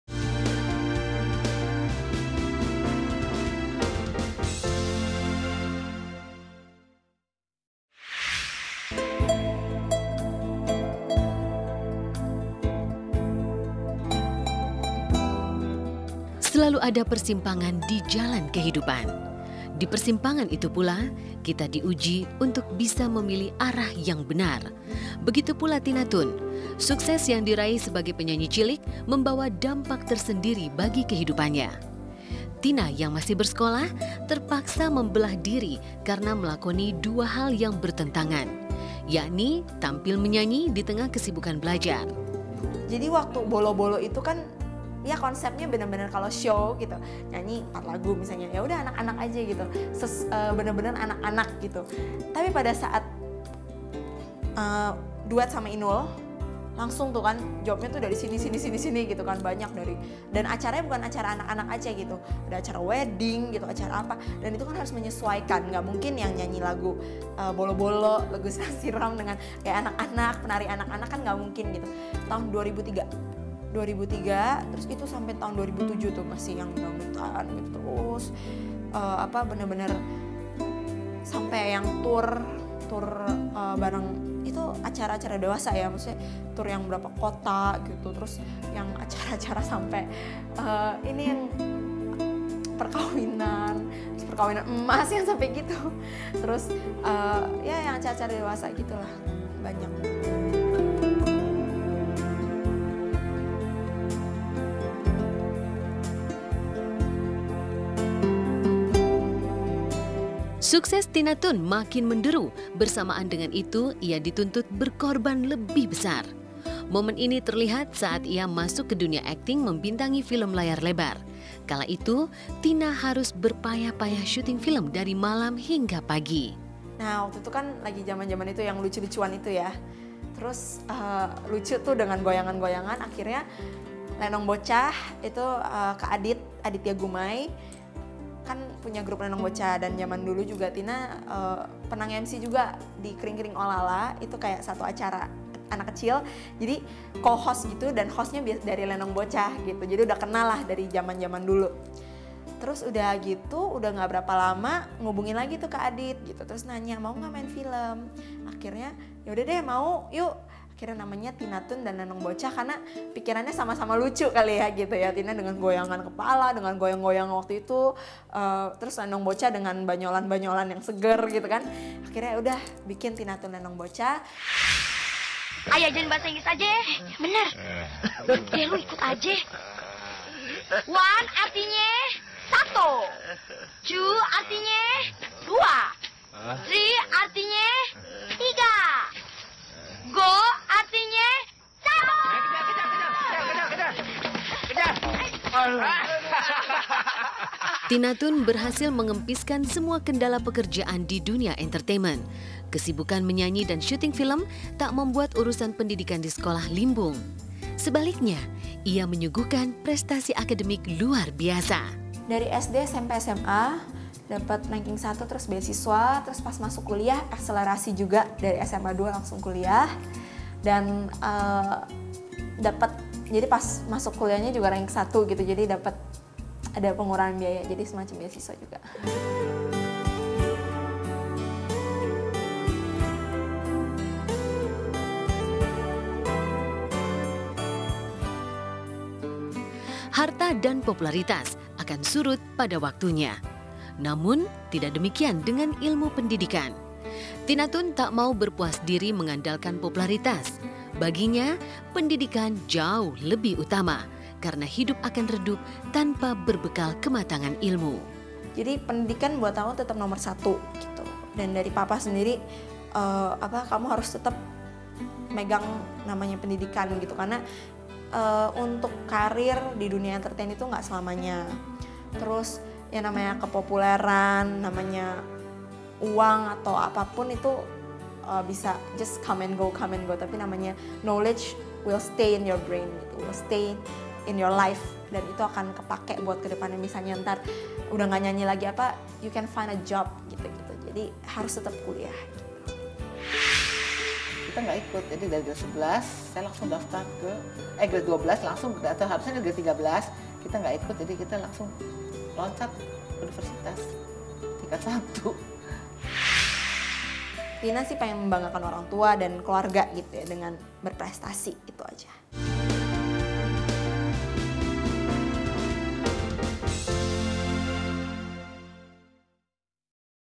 INSPIRASI SELEBRITI: TINA TOON (Part 3)
Bagaimana cerita hidup dan perjalanan karir Tina Toon? Inilah Inspirasi Selebriti